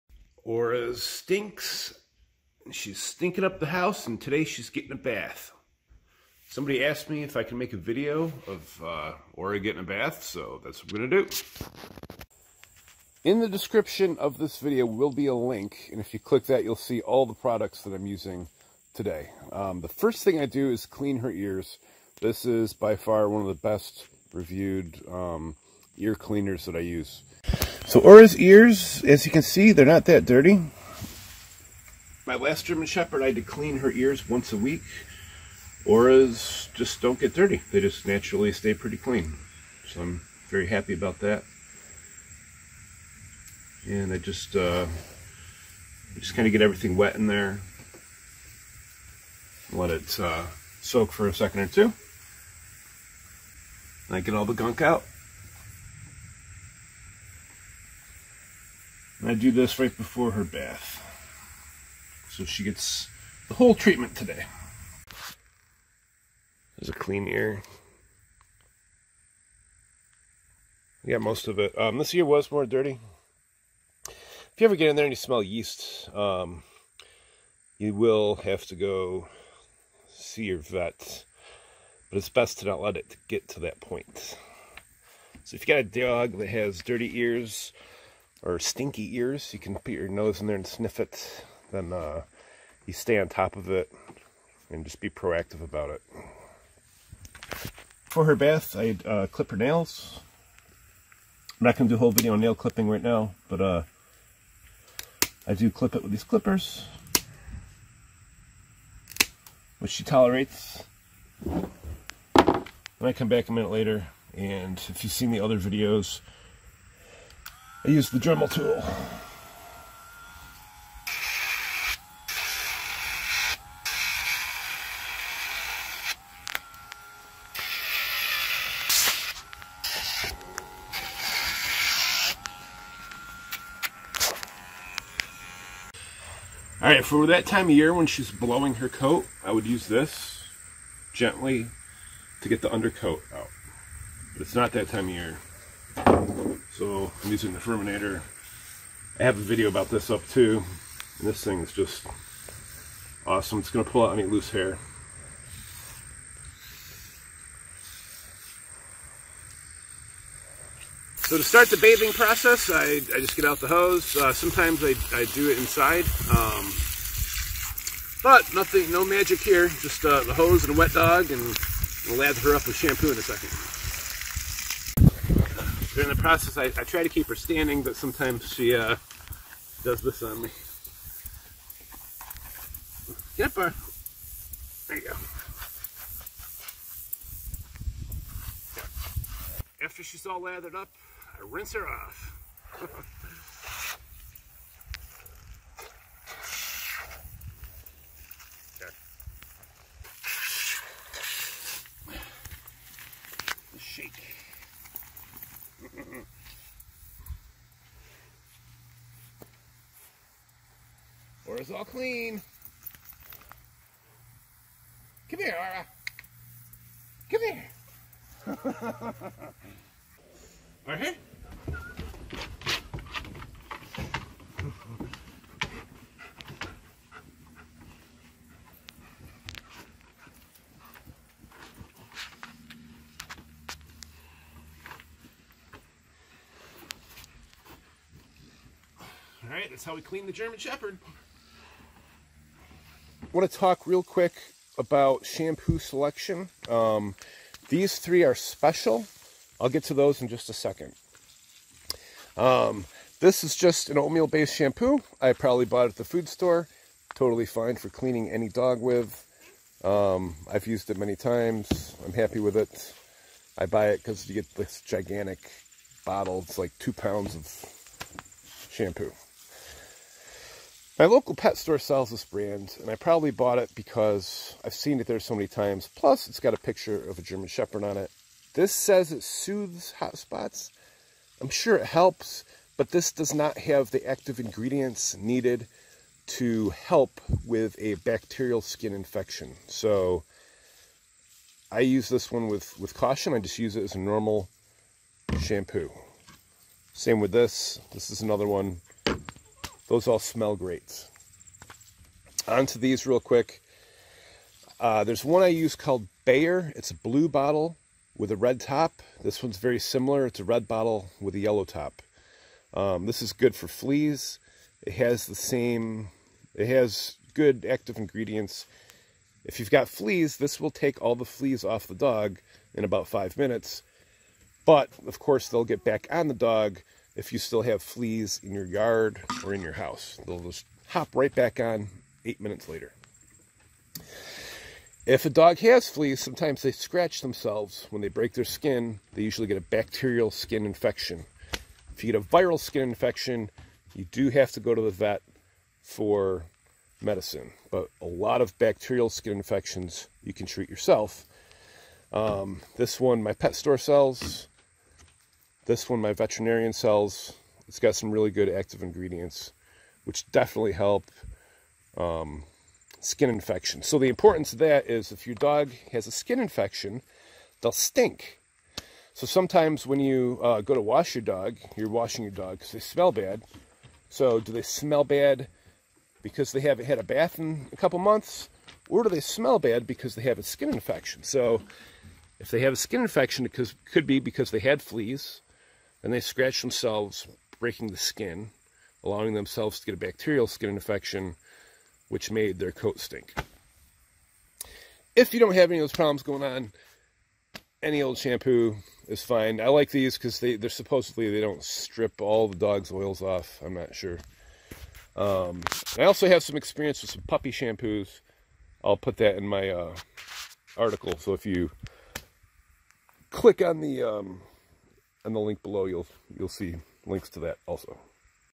Bath Time with our German Shepherd Dog Aura
- As someone requested - bath time!!
- This video is about how we bathe our German Shepherd Dog Aura. We usually start by cleaning her ears and trimming her nails. Then we get over to the hose where the cleaning process begins!